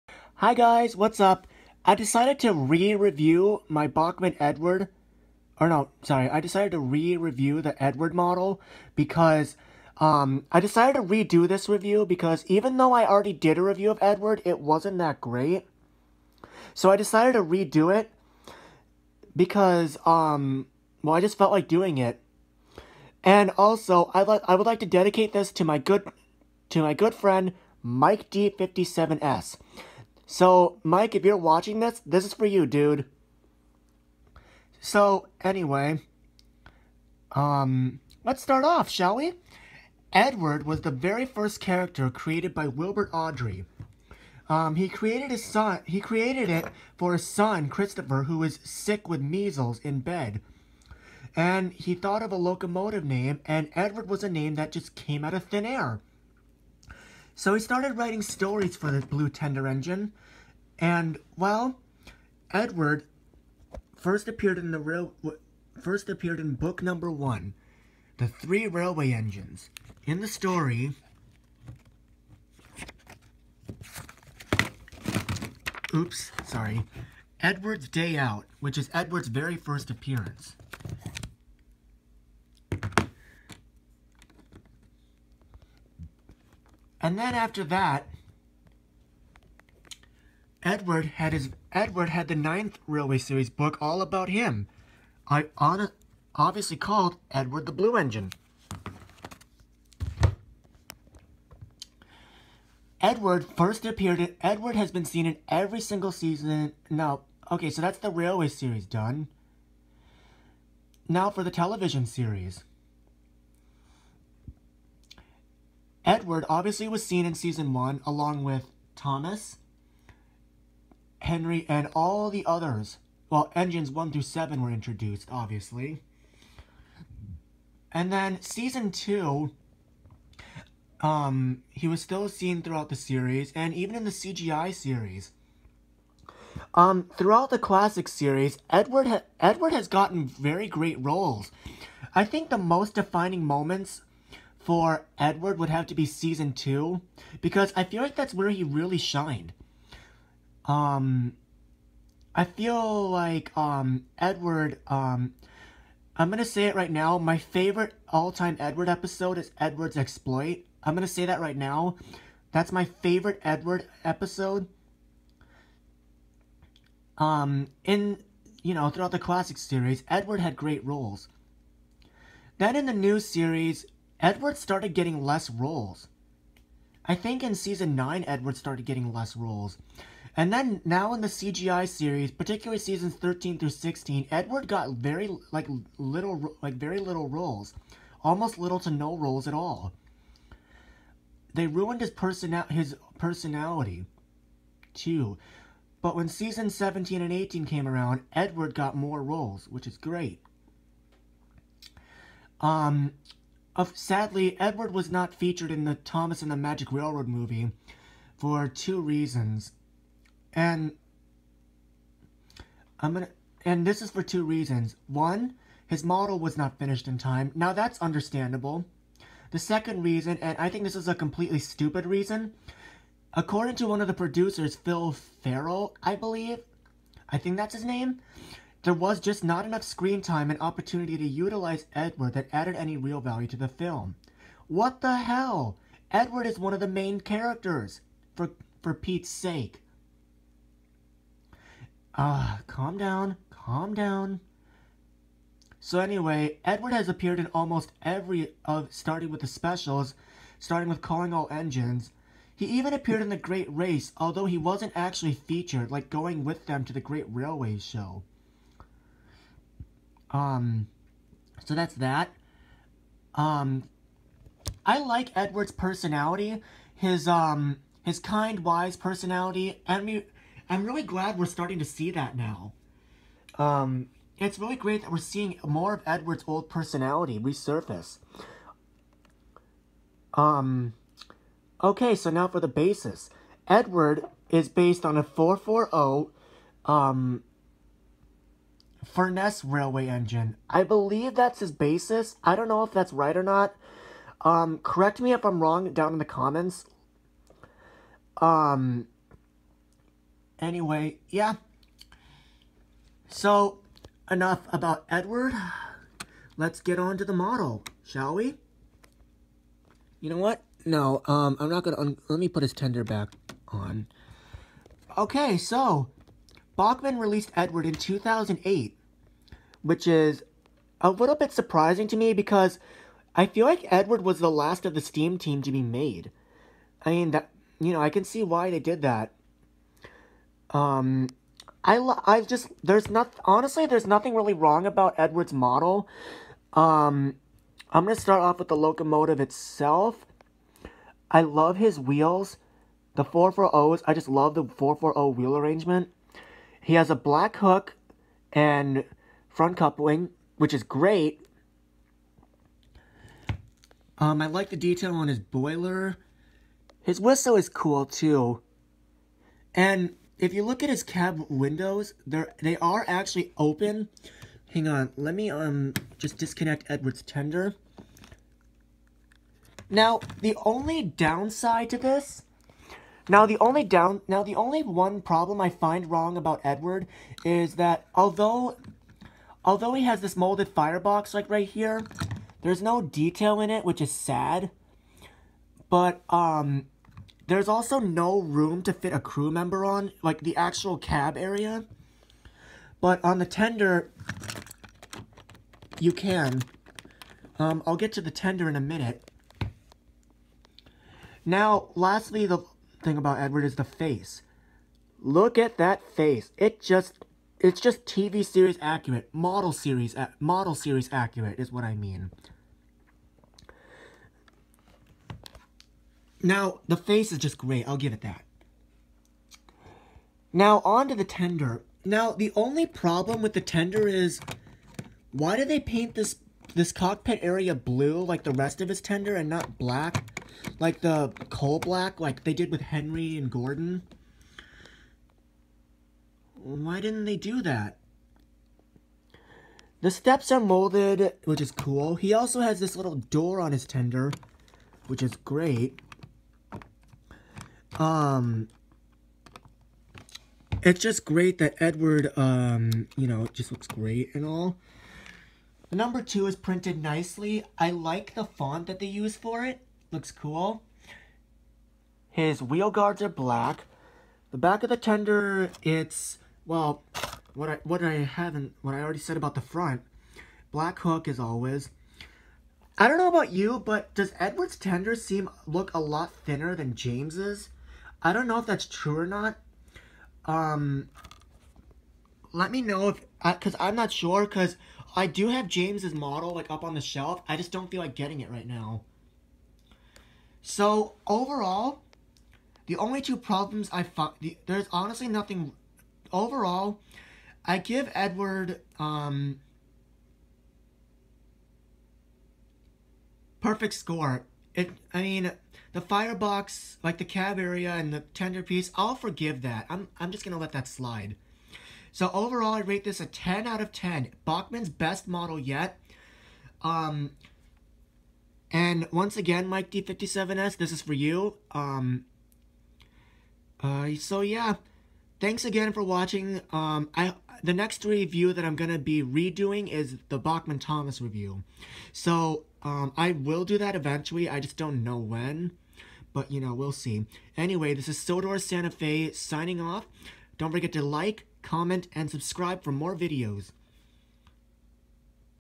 Bachmann Edward review (Redone) dedicated to MikeD57s
I decided to redo my Bachmann Edward review and this redone review is dedicated to my good friend MikeD57S